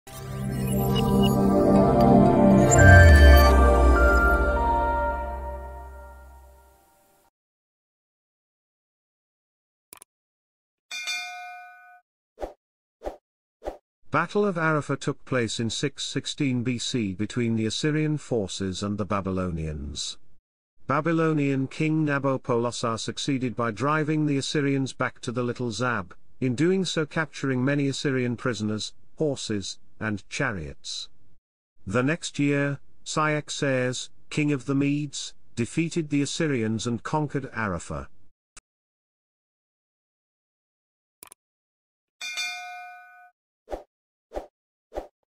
Battle of Arrapha - Wikipedia article
Video summary:
Battle of Arrapha took place in 616 BC between the Assyrian forces and the Babylonians.